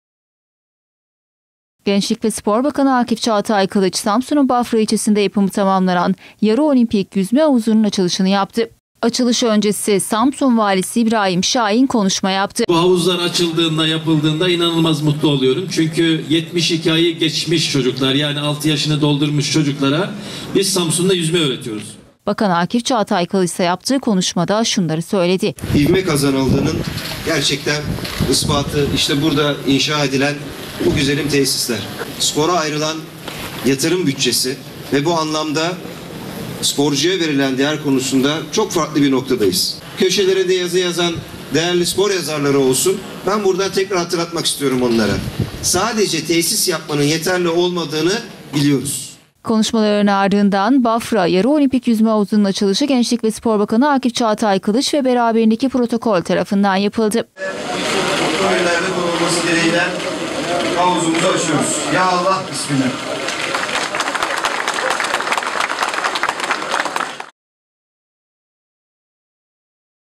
SAMSUN HAVUZ AÇILIŞI
Gençlik ve Spor Bakanı Akif Çağatay Kılıç, Samsun'un Bafra ilçesinde yapımı tamamlanan bin kişilik yarı olimpik havuzun açılışını yaptı.